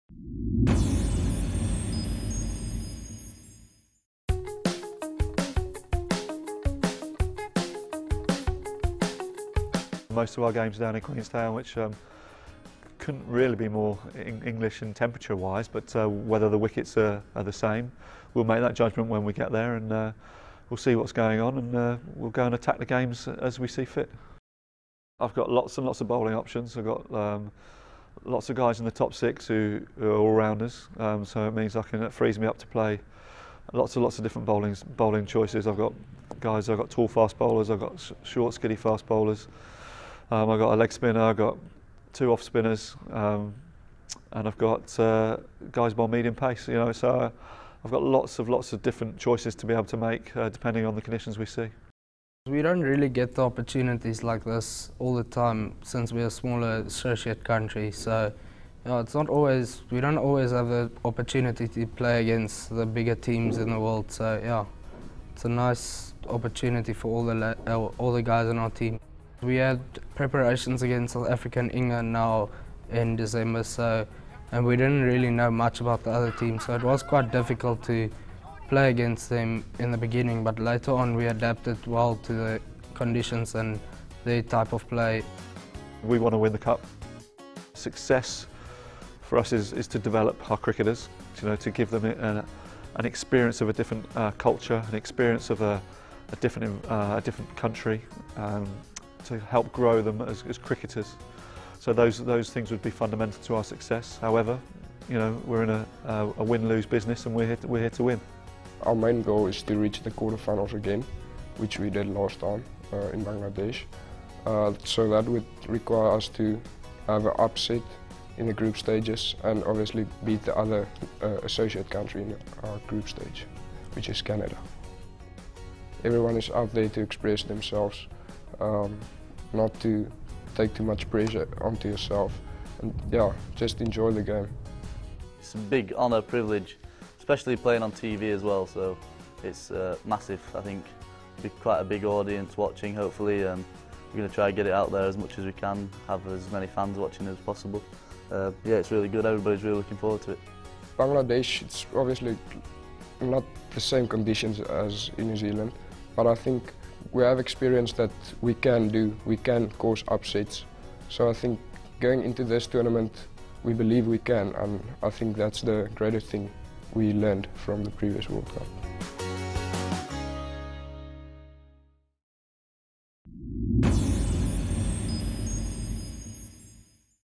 Match Preview: England U19s v Namibia U19s
England and Namibia U19s square off at one of the most beautiful cricket grounds on earth. Watch our preview and follow the live blog here!